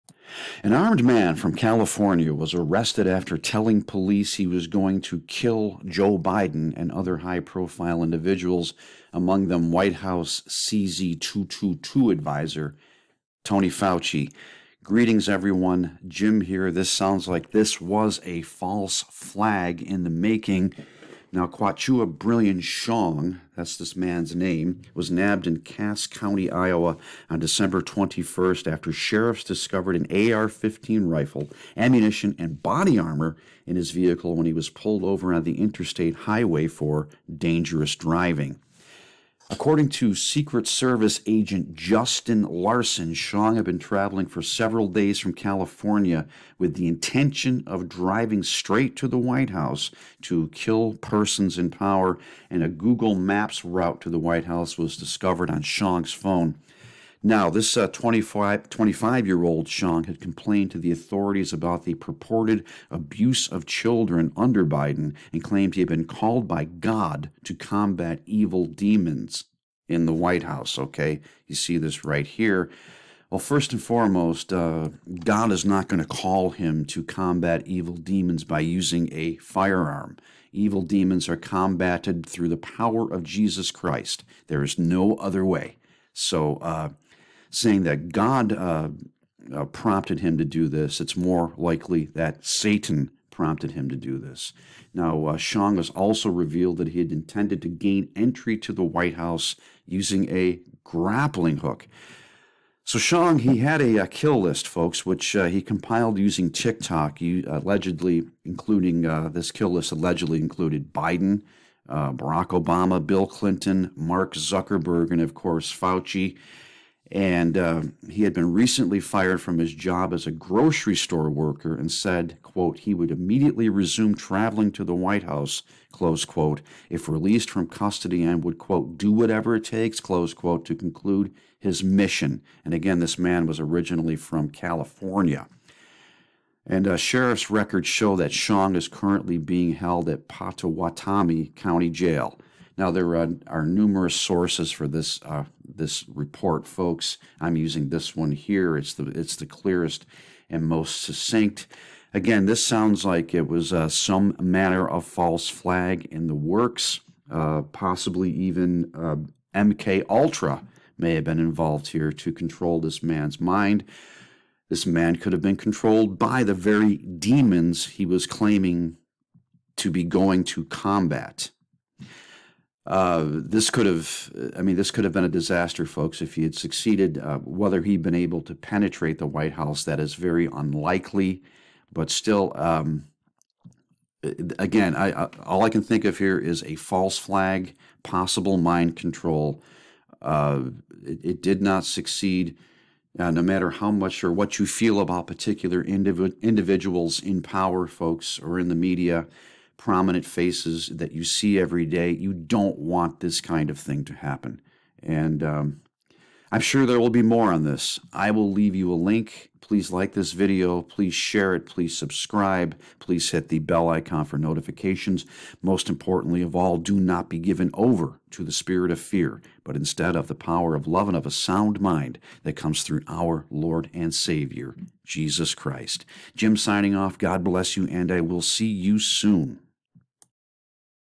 Armed Man Arrested on Way to White House to Rid 'Evil Demons'
An armed man from California was arrested after telling police officers he was going to kill US President Joe Biden and other high-profile individuals,
among them White House Covid adviser Anthony Fauci.

Check out my End-Times Fiction Book Series THE WAYFARERS available in Kindle and Print on Amazon